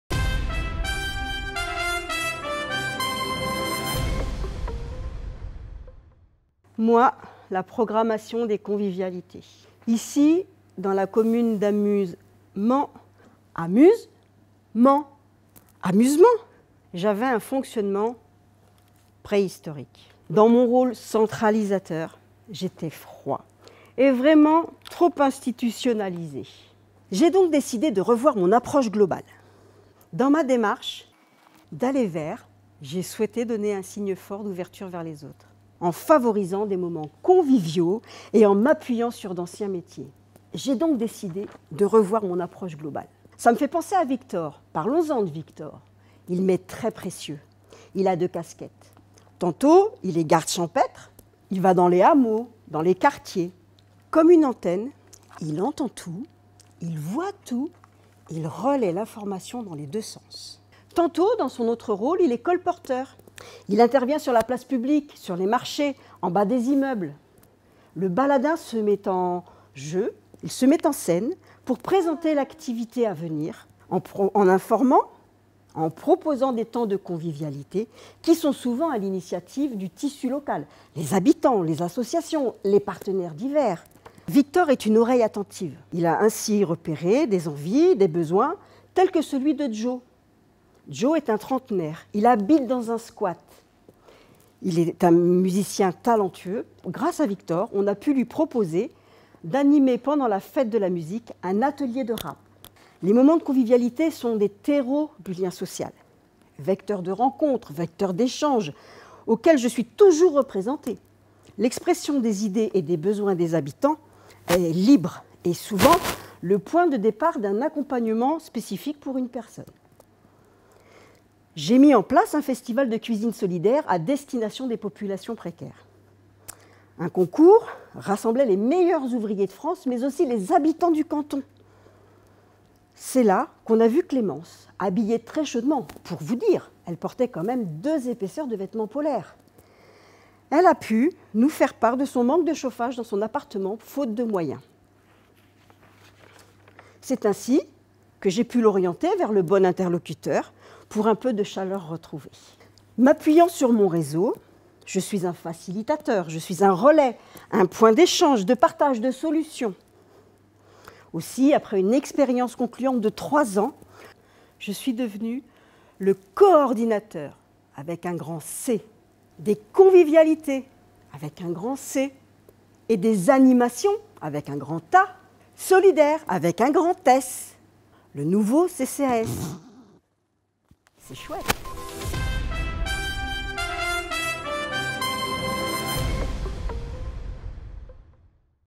Le CCAS en 2050 - Le programmateur de convivialités
Les actions imaginées au cours des ateliers du cycle Le CCAS en 2050, mené par l'Unccas avec l'agence Vraiment Vraiment et Les Beaux Yeux,  permettent de faire émerger différentes ﬁgures du CCAS/CIAS du futur, susceptibles d’inspirer autant de stratégies, de choix d’orientation et d’axes de développement. Ces ﬁgures ne s’excluent pas et peuvent co-exister, s’articuler et se compléter.

Découvrez le "programmateur de convivialités" imaginé par les participants à l'atelier du Mans d'après les actions suivantes :